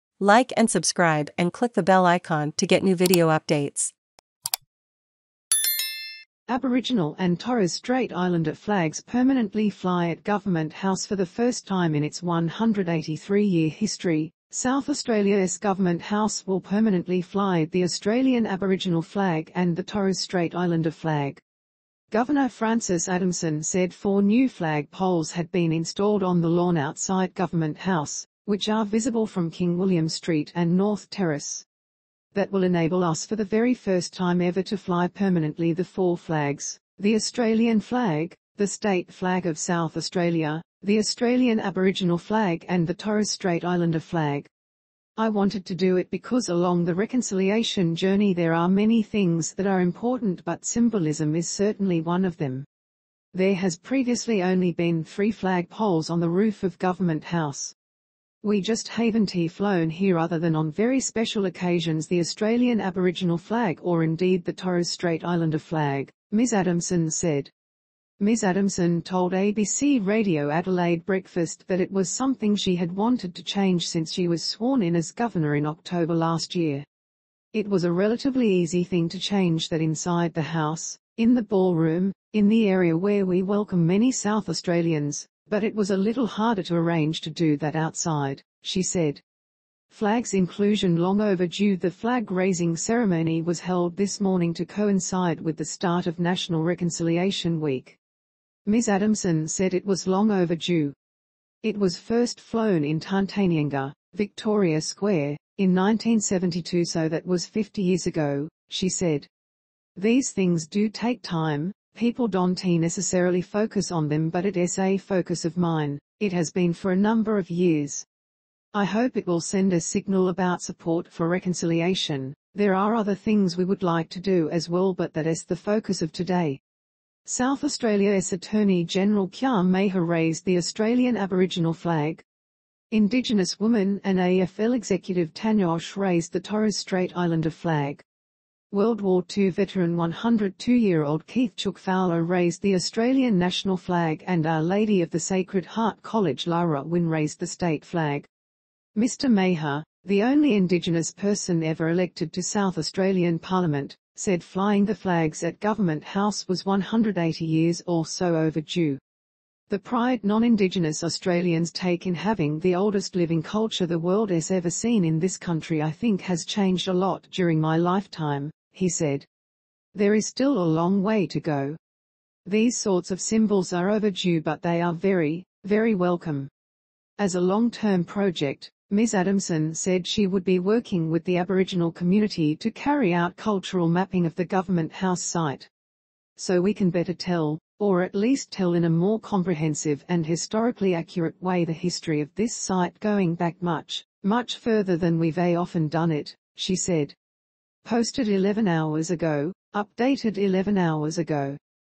ABORIGINAL AND TORRES STRAIT ISLANDER FLAGS PERMANENTLY FLY AT GOVERNMENT HOUSE
For the first time in its 183 year history. South Australia's Government House will permanently fly the Australian Aboriginal flag and the Torres Strait Islander flag. Governor Frances Adamson said four new flag poles had been installed on the lawn outside Government House. which are visible from King William Street and North Terrace. 'That will enable us for the very first time ever to fly permanently the four flags – the Australian flag. the state flag of South Australia. the Australian Aboriginal flag and the Torres Strait Islander flag.' 'I wanted to do it because along the reconciliation journey there are many things that are important but symbolism is certainly one of them.' There has previously only been three flag poles on the roof of Government House. 'We just haven't flown here other than on very special occasions the Australian Aboriginal flag or indeed the Torres Strait Islander flag.' Ms Adamson said. Ms Adamson told ABC Radio Adelaide Breakfast that it was something she had wanted to change since she was sworn in as Governor in October last year. 'It was a relatively easy thing to change that inside the house. in the ballroom. in the area where we welcome many South Australians. but it was a little harder to arrange to do that outside.' she said. Flags inclusion 'long overdue' The flag raising ceremony was held this morning to coincide with the start of National Reconciliation Week. Ms Adamson said it was 'long overdue'. 'It was first flown in Tarntanyangga — Victoria Square — in 1972 so that was 50 years ago.' she said. 'These things do take time. people don't necessarily focus on them but it's a focus of mine. it has been for a number of years. 'I hope it will send a signal about support for reconciliation. there are other things we would like to do as well but that's the focus of today.' South Australia's Attorney General Kyam Maher raised the Australian Aboriginal flag. Indigenous woman and AFL executive Tanya Hosch raised the Torres Strait Islander flag. World War II veteran 102 year old Keith 'Chook' Fowler raised the Australian national flag and Our Lady of the Sacred Heart College Lara Nguyen raised the state flag. Mr Maher — the only Indigenous person ever elected to South Australian parliament — said flying the flags at Government House was '180 years or so overdue'. 'The pride non indigenous Australians take in having the oldest living culture the world's ever seen in this country I think has changed a lot during my lifetime.' he said. 'There is still a long way to go. These sorts of symbols are overdue but they are very. very welcome.' As a long term project. Ms Adamson said she would be working with the Aboriginal community to carry out 'cultural mapping' of the Government House site. 'So we can better tell. or at least tell in a more comprehensive and historically accurate way the history of this site going back much. much further than we've often done it.' she said. Posted 11h ago. updated 11h ago